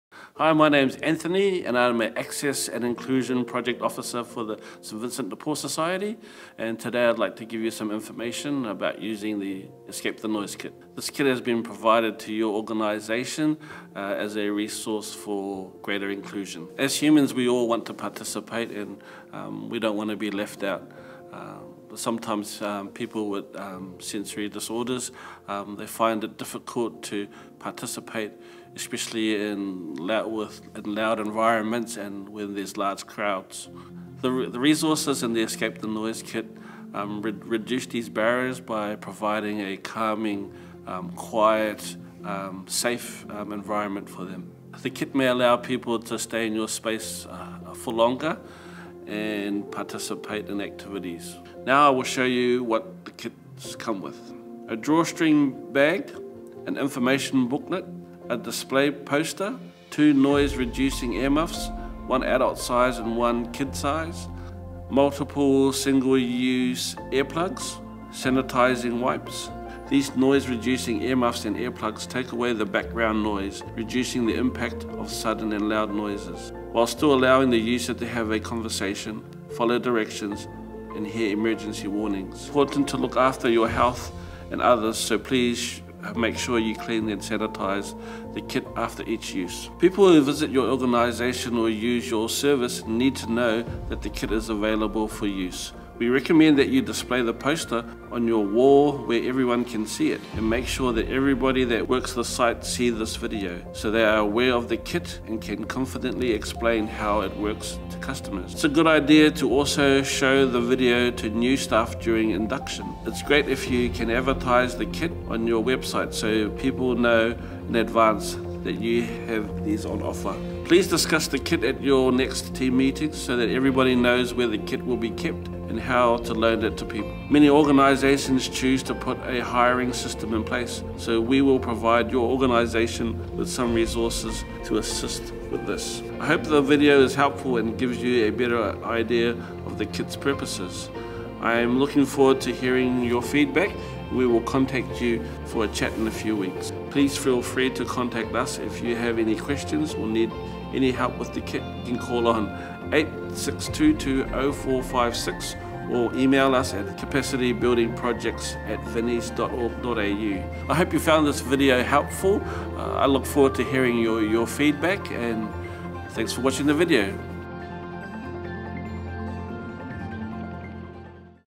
Escape the Noise Kit - Vinnies Access & Inclusion
Vinnies is helping local organisations and businesses to be more inclusive. We’re providing kits, free of charge, which allow people with sensory disorders to participate in environments which may usually not be accessible. These kits help reduce barriers to access, providing a calming, quiet and safe environment so that everyone has a chance to get involved.